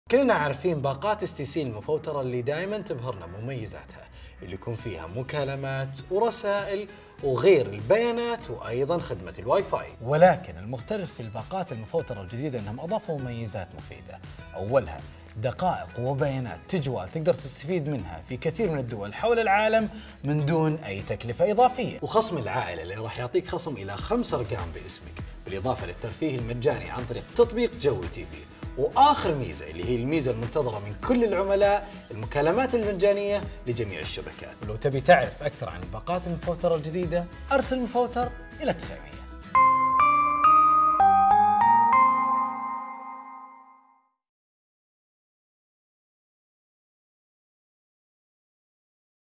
STC - مميزات باقات المفوتر الجديدة
تعرّف أكثر على المميزات الجديدة في كل باقة
للمزيد، أرسل "مفوتر" إلى 900 أو تفضّل بزيارة الرابط
فيديو من شركة الاتصالات السعودية STC
Video by Saudi Telecom Company – STC .